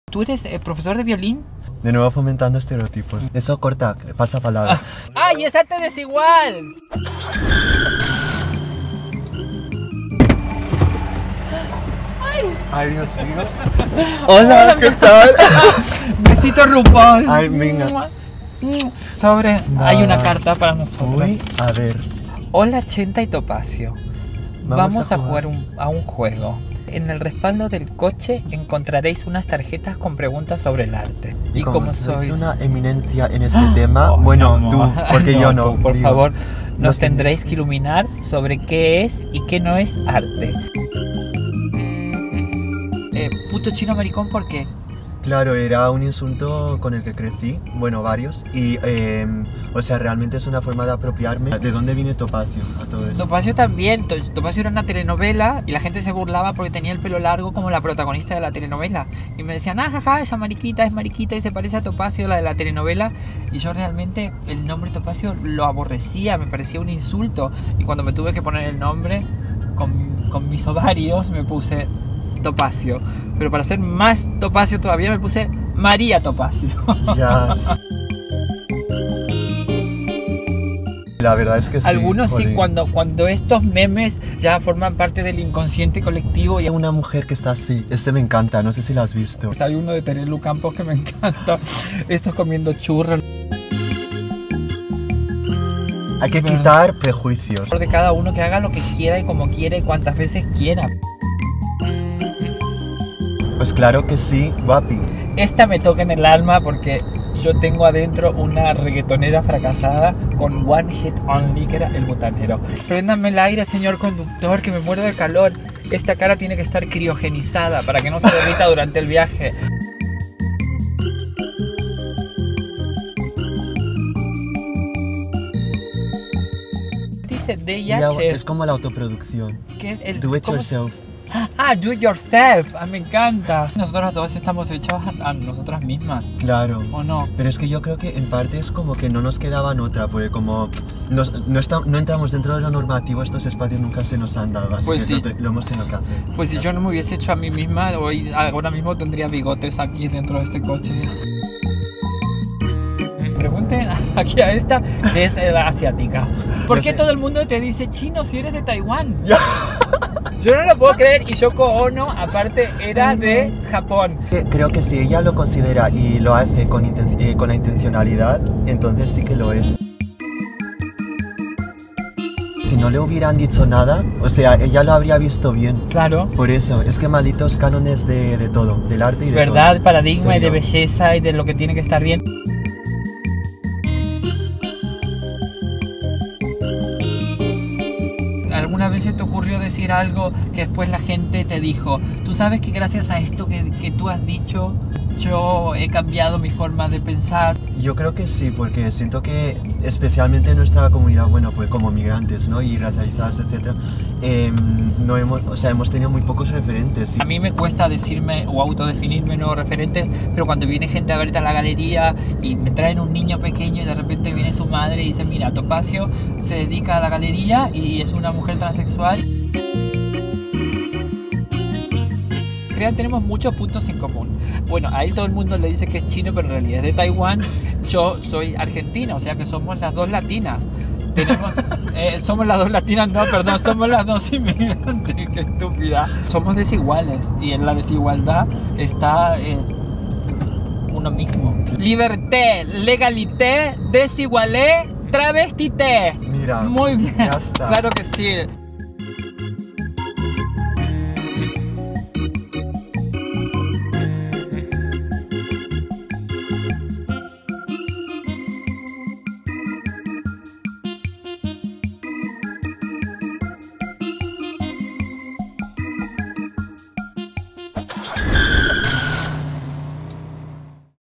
DRIVE ME TO - Topacio Fresh & Chenta Tsai | Desigual
Topacio Fresh (singer, actress and transgender activist from Argentina) is in a car with Chentai Tsai (aka @putochinomaricon), multidisciplinary artist. What will happen next?! Find out in this video.

Chenta Tsai (@putochinomaricon) and Topacio (@topaciofresh) will be enlightening us in the ways of art. Do we all have an artist inside? Is 'Ecce Homo de Borja' the greatest artwork ever? We don't know, but surely our passengers do...

Drive Me To is an initiative from Desigual taking place at the 69th edition of MBFWMadrid in which different personalities will talk (and have fun doing so) about different current issues. Fashion, art, future and authenticity will be the starting points of these talks that will take place in the back seat of a Mercedes-Benz Class A.

Discover the looks Topacio y Chenta are wearing on our